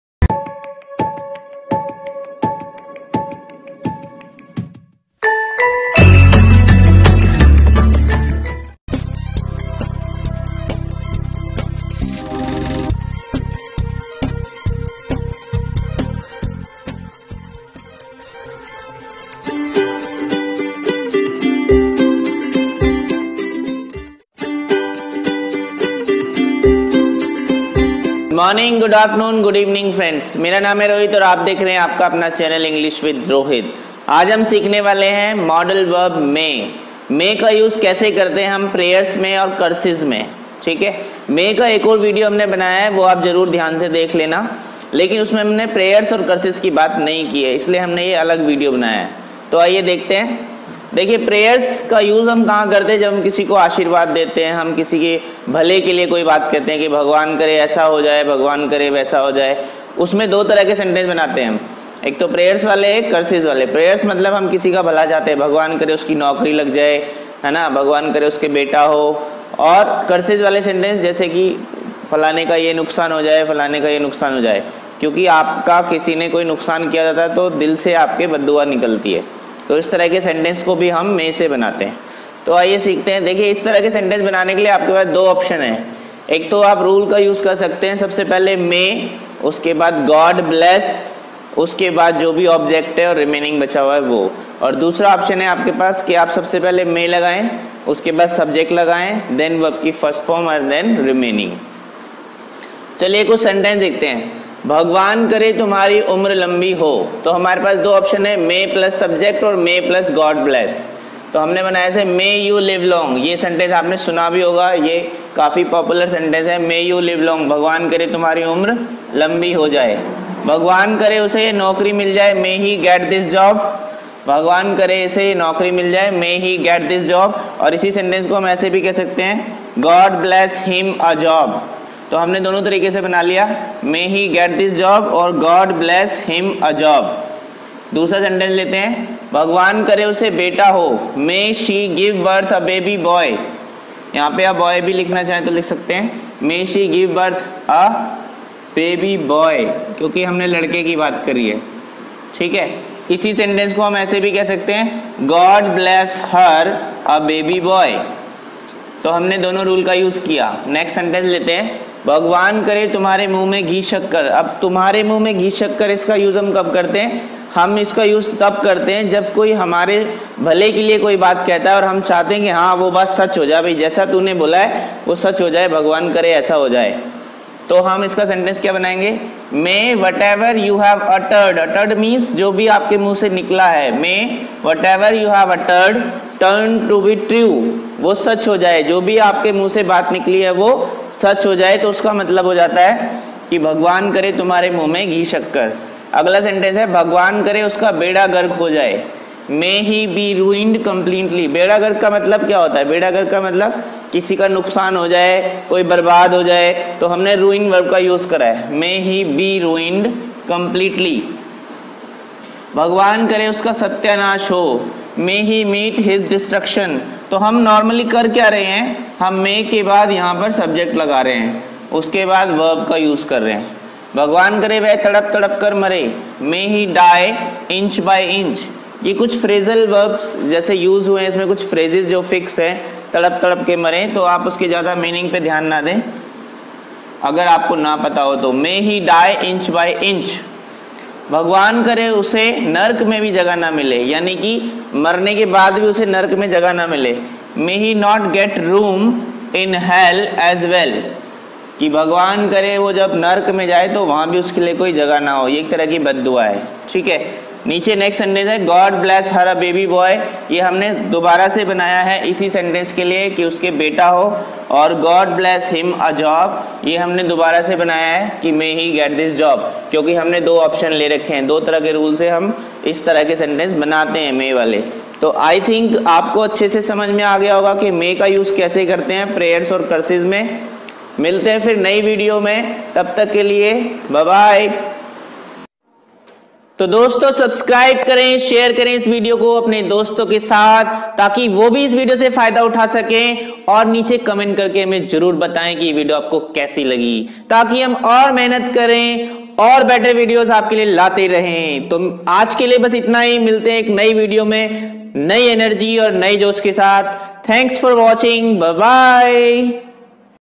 Modal verb 'May' | modal verbs in English grammar
We provide educational, English learning and motivational videos.

In this video you will learn modal verb - may.  How to use may in curses and blessing sentences.  How to curse and bless somebody in English language.